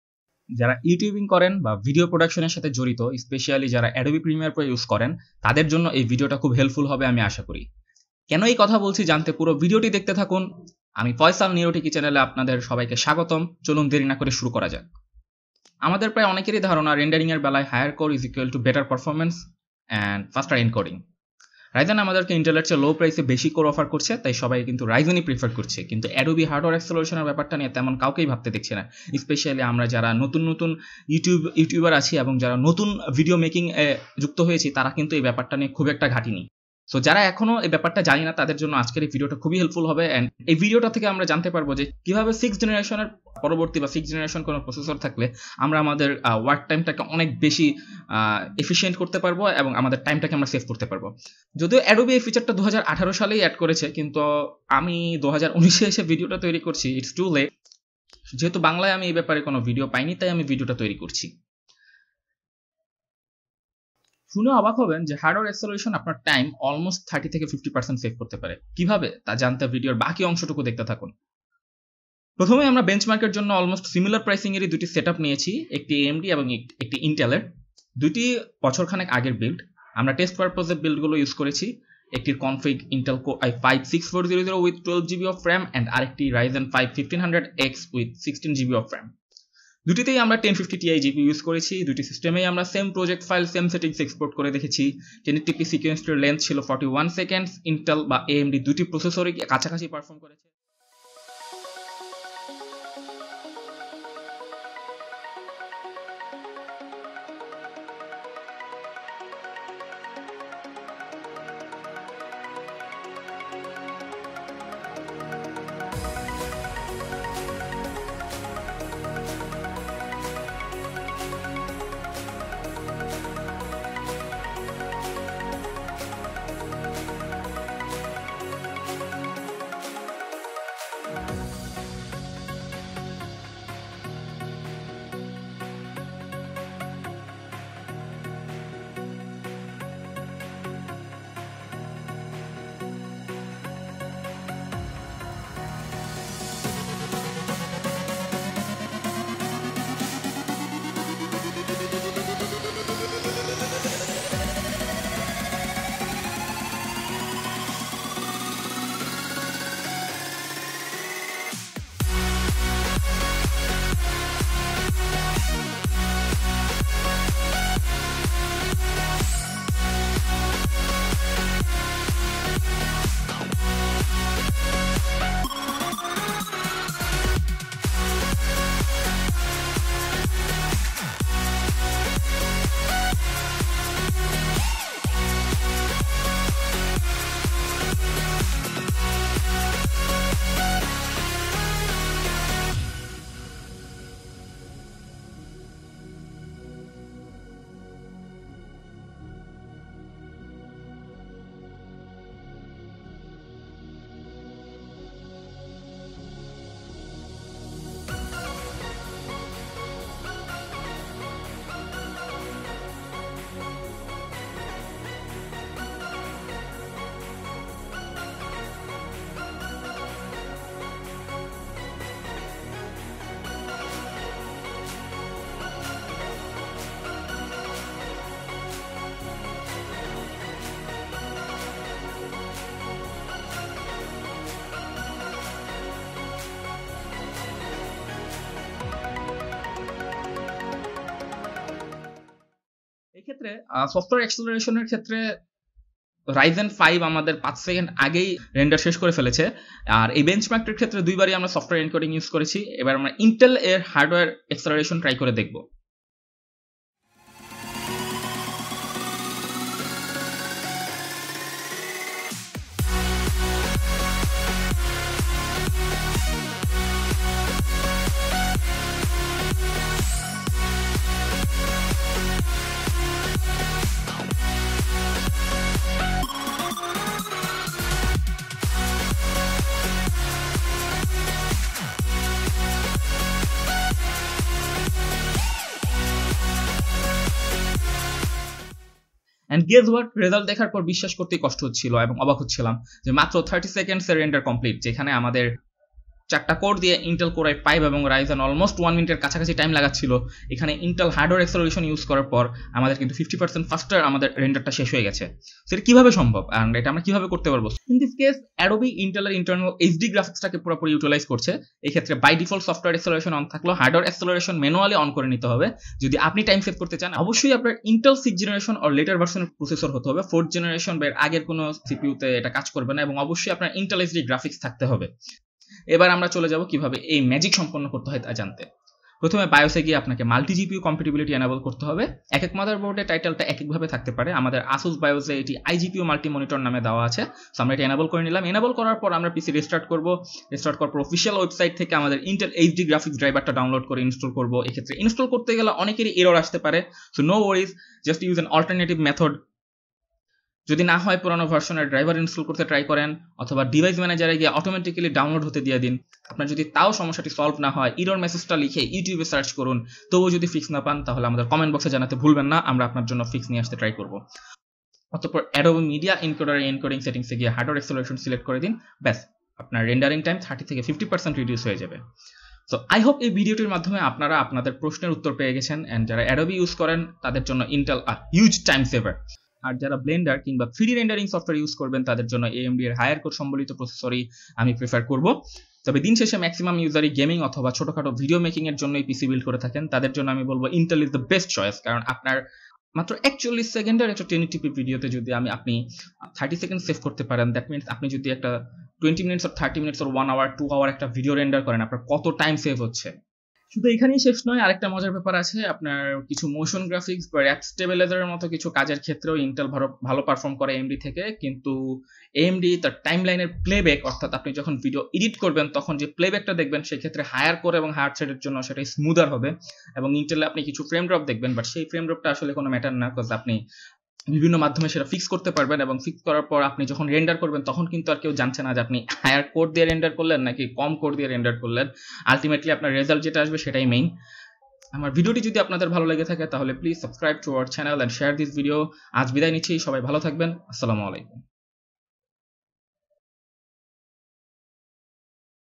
Intel vs Ryzen For Adobe | Intel is better than ryzen?!! | Ft. Hardware Acceleration - Bangla
ইন্টেল আর রাইজেন এর মধ্যে কোন প্রসেসর ভিডিও এডিটিং এ বেস্ট এবং কেন ?

4:48 - Main conclusion